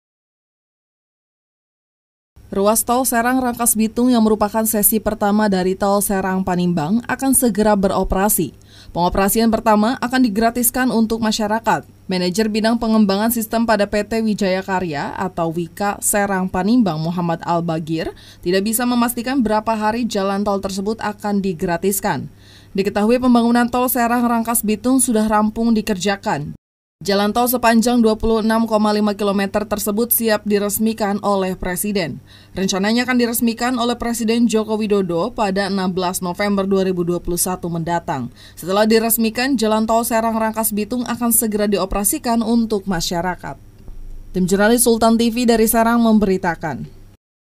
Tol Serang - Rangkasbitung Akan Digratiskan - Berita Sultan
Ruas Tol Serang-Rangkasbitung yang merupakan sesi pertama dari Tol Serang-Panimbang akan segera beroperasi. Pengoperasian pertama akan digratiskan untuk masyarakat.

Manager Bidang Pengembangan Sistem pada PT Wijaya Karya (Wika) Serang-Panimbang, Muhammad Albagir tidak bisa memastikan berapa hari jalan tol tersebut akan digratiskan.

Diketahui, pembanguan tol Serang-Rangkasbitung sudah rampung dikerjakan. Jalan tol sepanjang 26,5 kilometer tersebut siap diresmikan oleh Presiden.